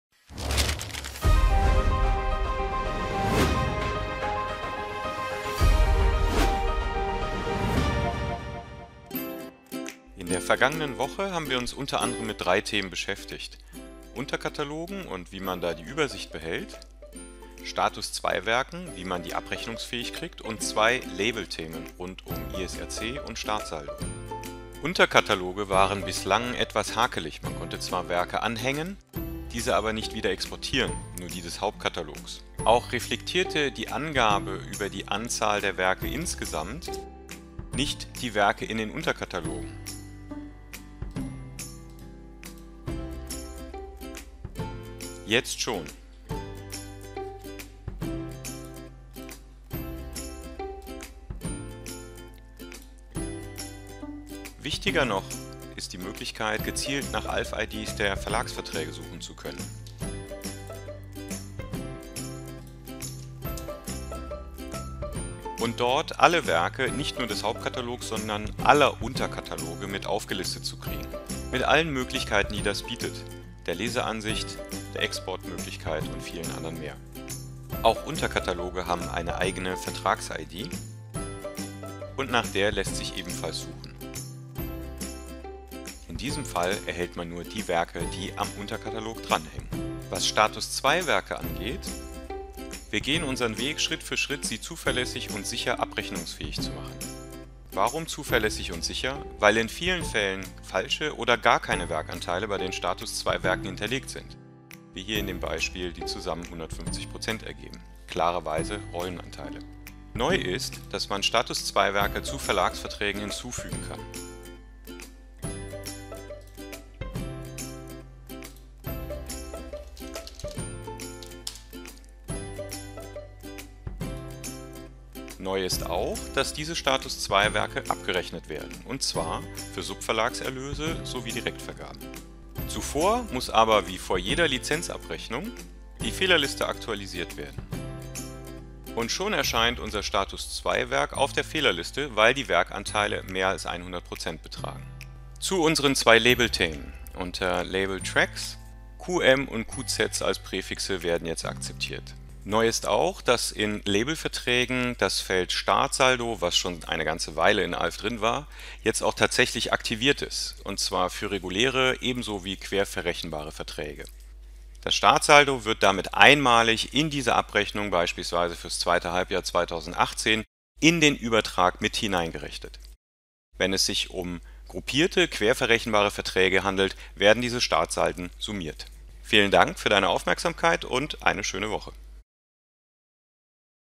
ALV: Neu in KW 34 mit ISRC, Status=2 Werken, Werk-Suche, Startsaldo in Labelverträgen
In aller Kürze, die wichtigsten Neuerungen der letzten Woche rund um Dein ALV.

Neu in KW 34:
Label: ISRC mit Gebietscodes QM und QZ (eigentlich US)
Verlag: Werk-Suche mit "CP"-ALVID's inkl. Unterkataloge